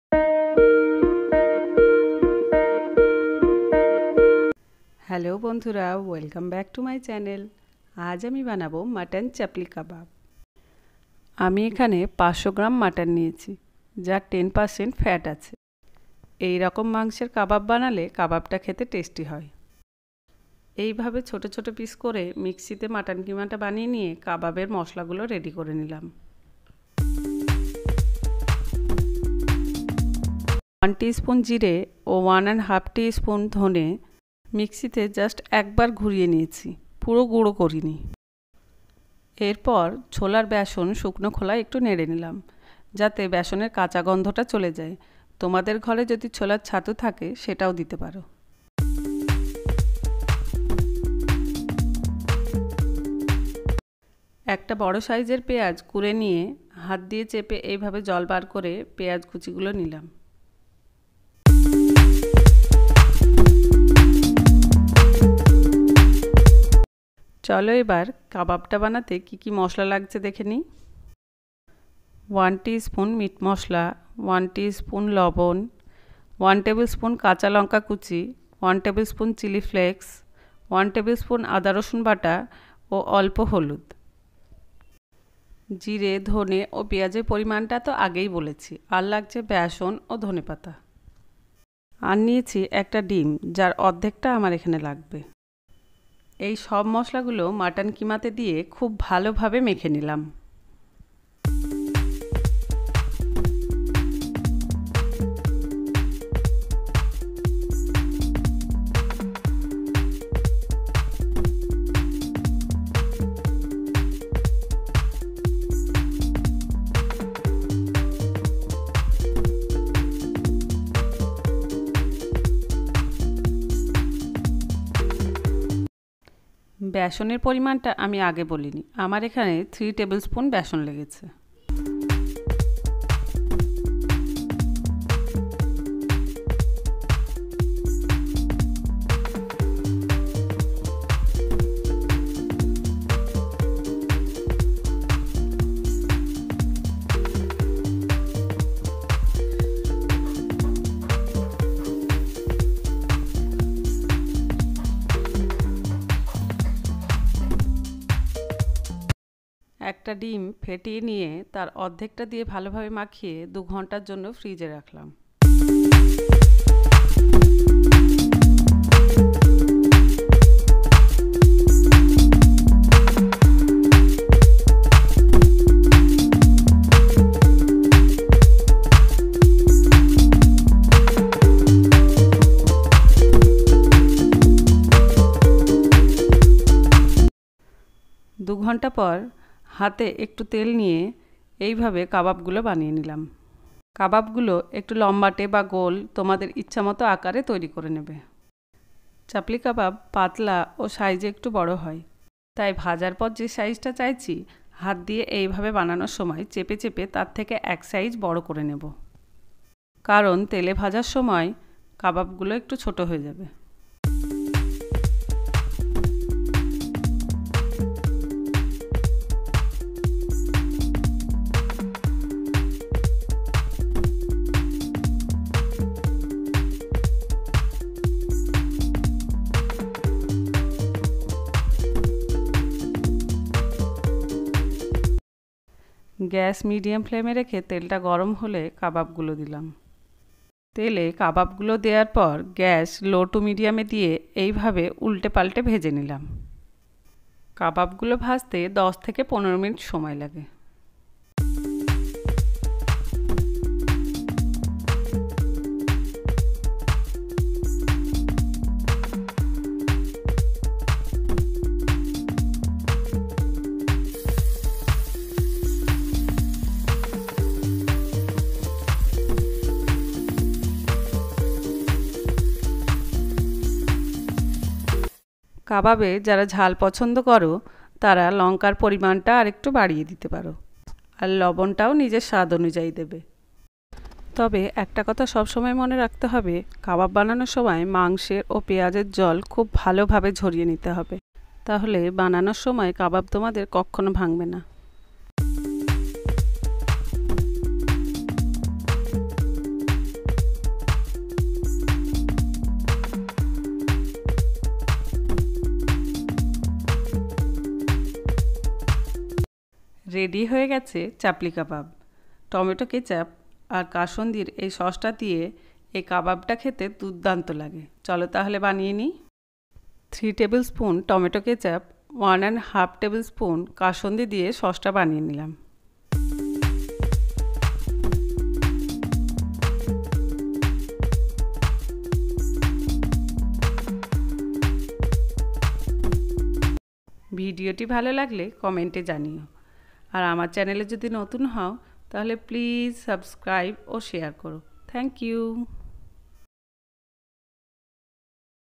Chapli kabab recipe/easy mutton kabab recipe
ingredients.

mutton 500 grm
onion 1big size
jeera 1 tbsp
coriander 1and 1/2tbsp
green chilli 1 tbsp
meat masala 1 tea spoon
 chickpea besan 3 tbsp
ginger garlic paste 1tbsp
turmeric
coriander leaves
egg half